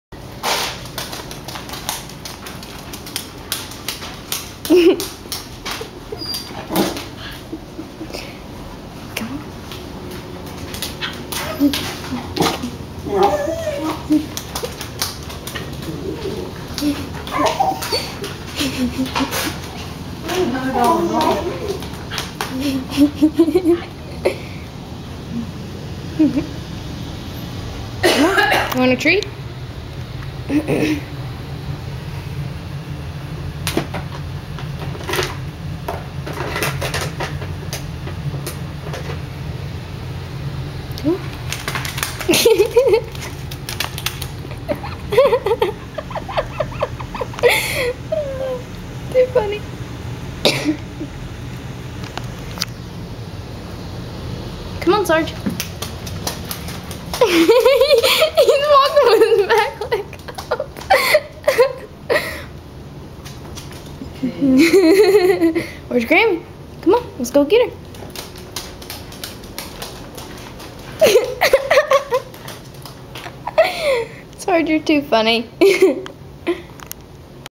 JACKASS KICK DOG
Sarge wearing boots and kicking back legs